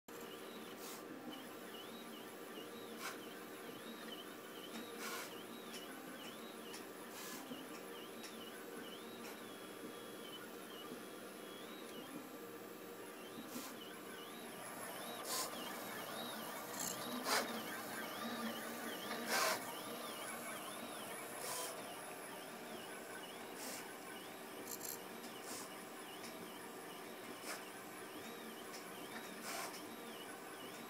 Printrbot variant running Marlin
Marlin is much smoother than Sprinter.

BTW, the extra noise is my Makerbot printing in the background. Marlin isn't louder than Sprinter.